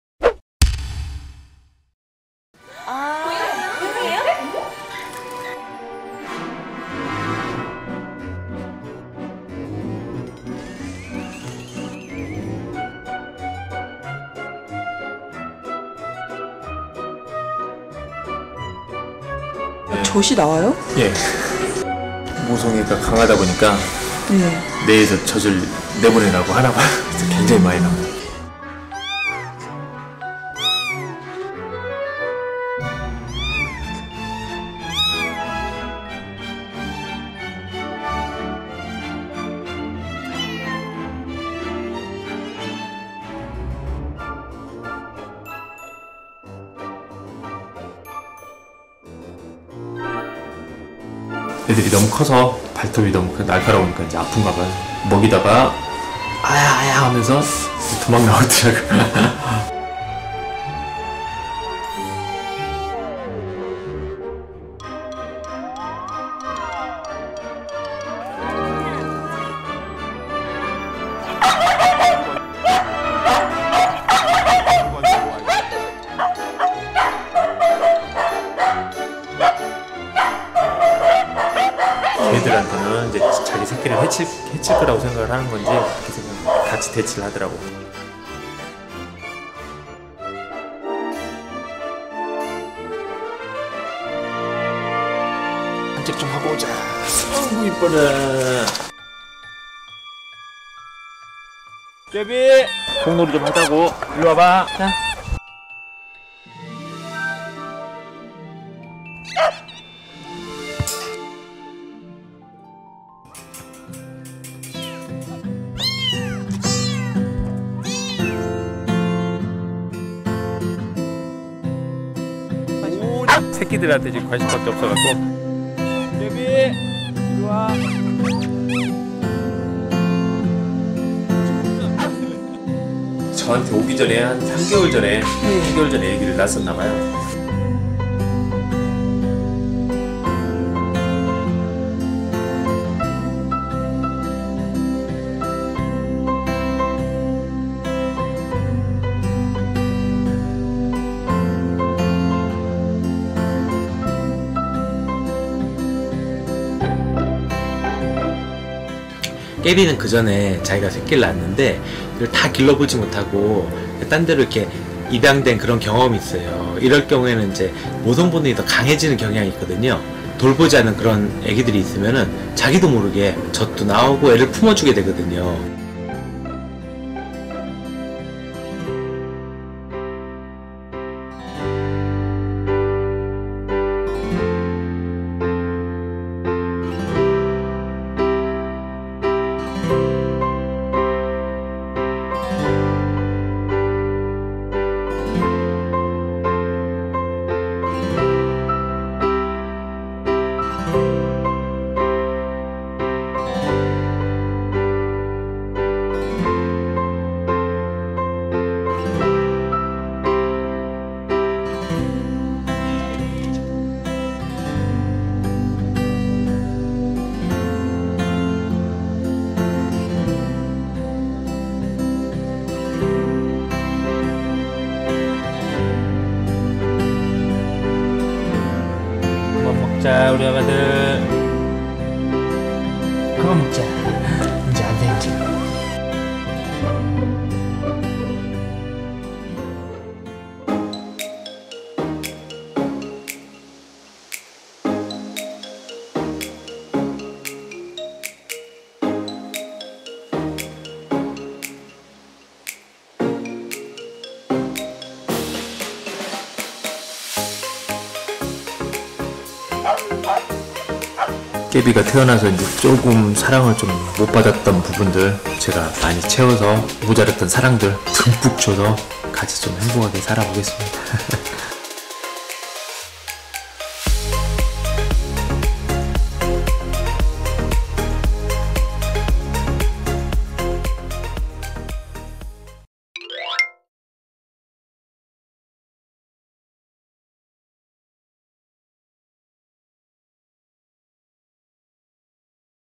A Dog Became A Mom Of Kittens After Losing Her Puppies | Kritter Klub
Kkaebi is a dog who tirelessly takes care of neglected kittens. Should Kkaebi continue to look after them though?